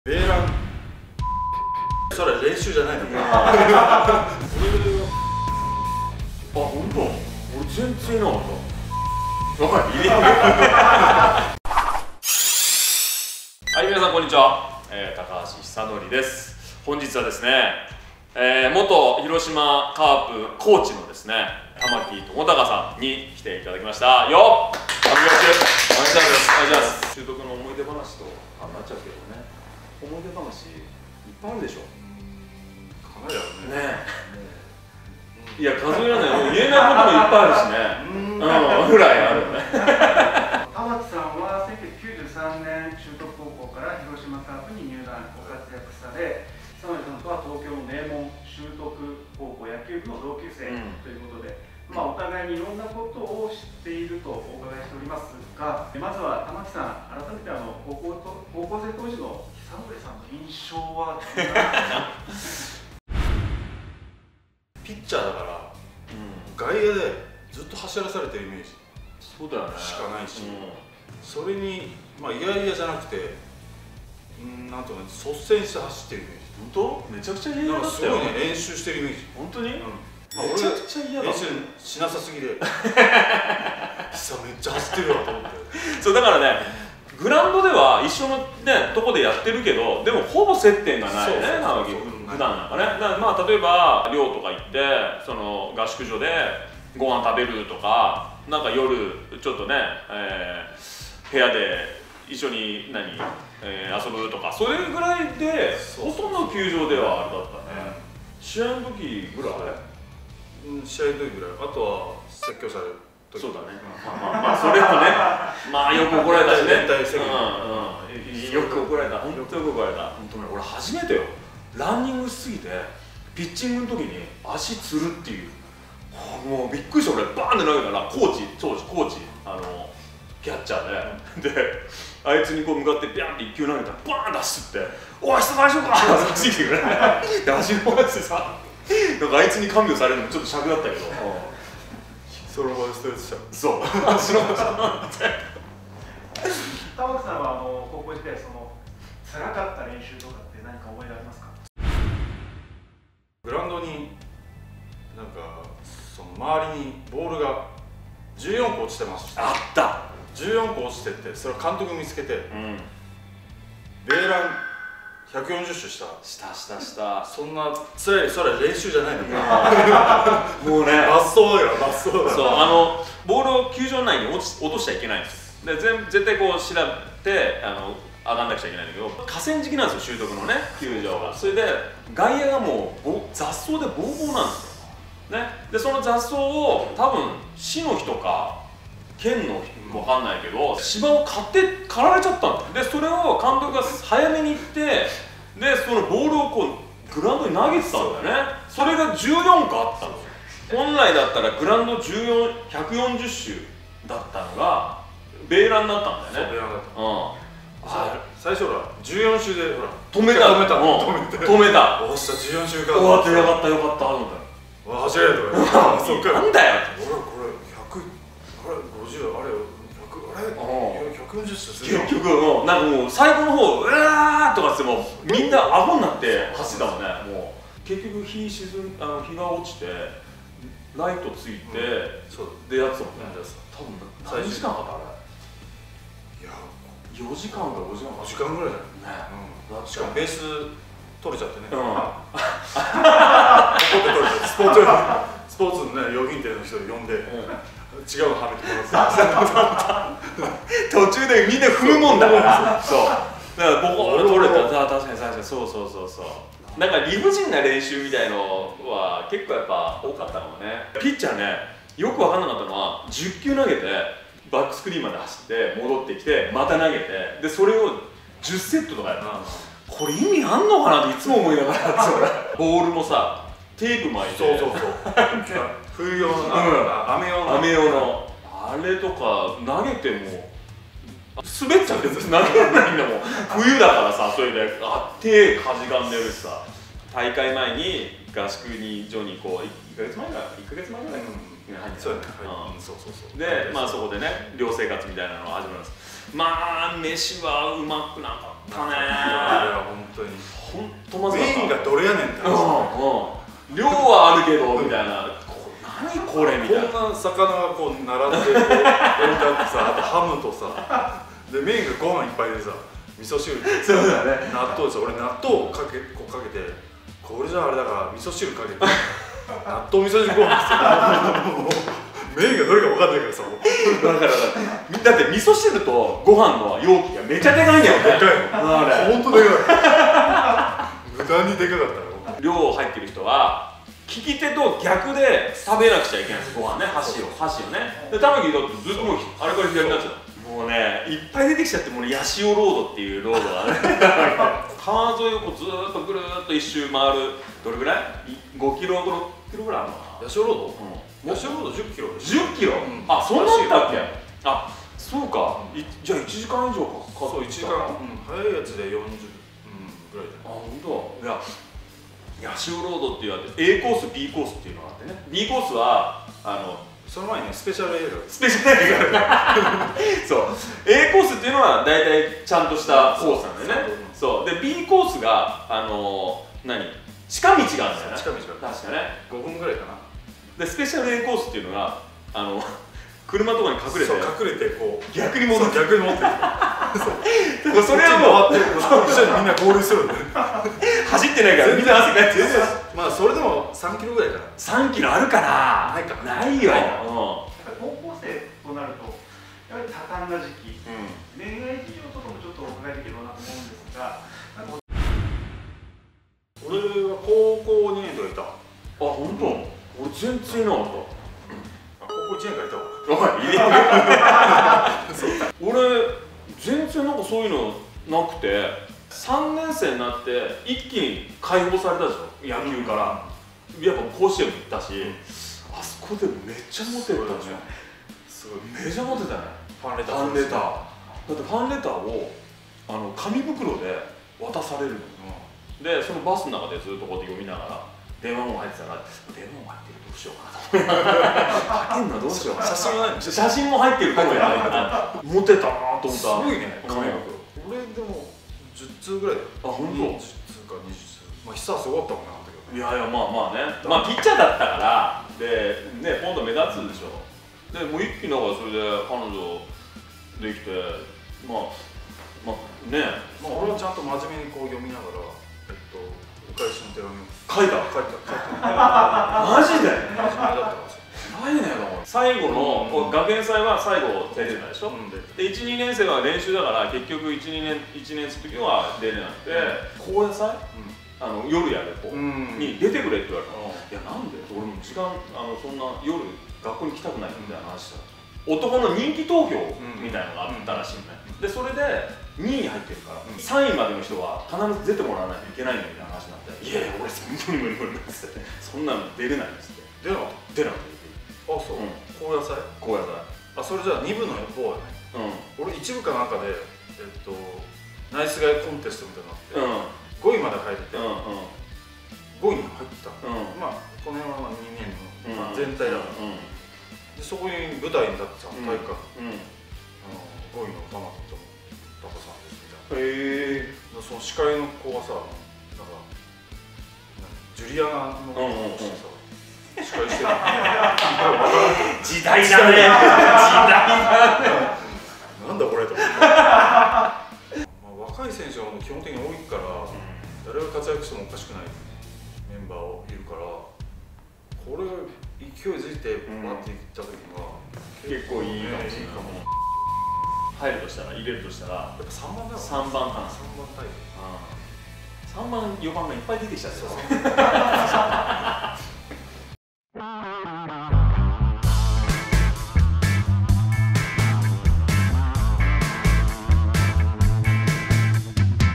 【青春】甲子園常連！修徳野球部の鬼練習！令和ではNG？尚成母校の仰天特訓事件簿とは⁉︎高校時代モテモテ？ファンレターが紙袋で…多感すぎる高校恋愛事情を野球部同級生と包み隠さず喋り尽くす！！
尚成の高校時代全てを知る男が登場！
春3回 夏5回 甲子園8回出場！ 尚成など13人のプロ野球選手を輩出した東京の名門・修徳高等高校野球部！今回は尚成の野球部時代の同級生で広島カープでは選手、コーチとして活躍された玉木朋孝さんと修徳高校野球部㊙︎対談！

「地獄の八潮ロード」…空前絶後の猛特訓！令和では考えられない？？甲子園常連の名門・修徳高校野球部の仰天練習とは⁉︎汗と泥に塗れた寮生活でも事件発生？寮のご飯は本当に〇〇すぎた…今だから楽しく話せる地獄の修徳野球部猛練習を尚成と玉木朋孝さんが語り尽くす！

尚成は高校時代モテモテだった？？ファンレターが紙袋で…野球も恋も大切な思春期真っ只中！高校時代の尚成を知る男、同級生玉木朋孝さんが、尚成の高校時代恋愛事情を包み隠さず喋り尽くす！！！

皆様是非楽しみながら最後までご覧ください！

今後も他では見られない・聞くことが出来ない
話や企画が沢山ありますので

―ーWEB・SNSーー
●オフィシャルウェブサイト